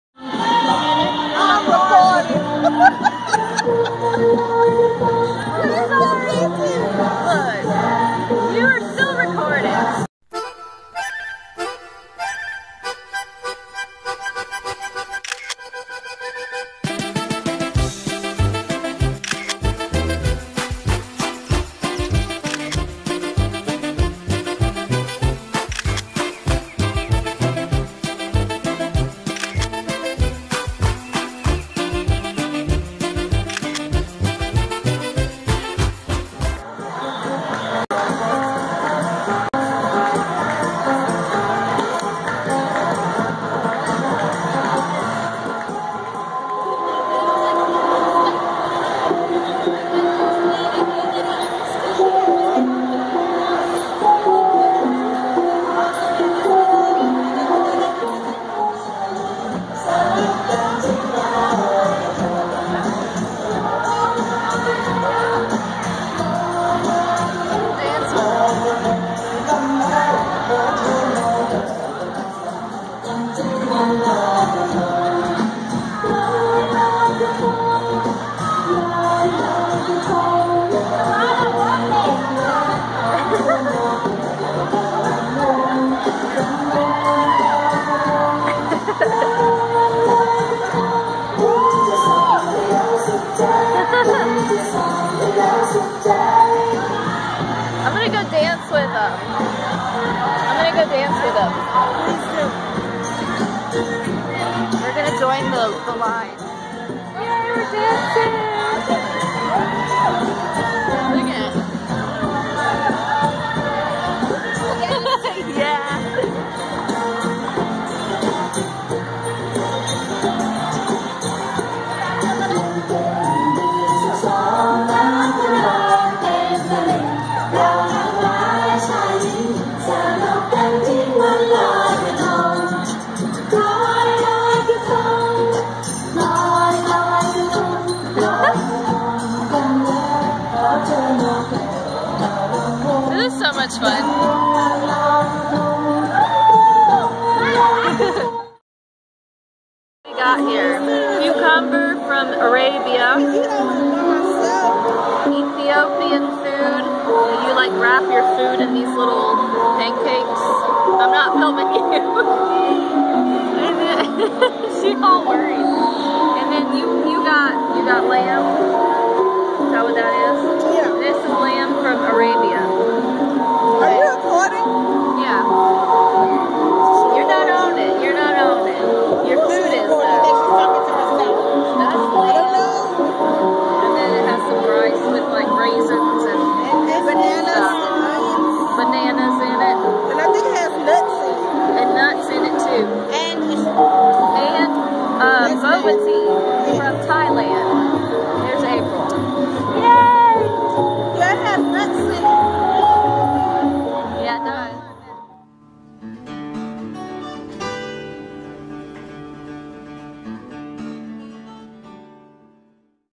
TRYING ETHIOPIAN FOOD AND ARABIC FOOD AT THE INTERNATIONAL FESTIVAL 2017
International Festival, Asian/Japanese culture dance, Arabic and Ethiopian Food!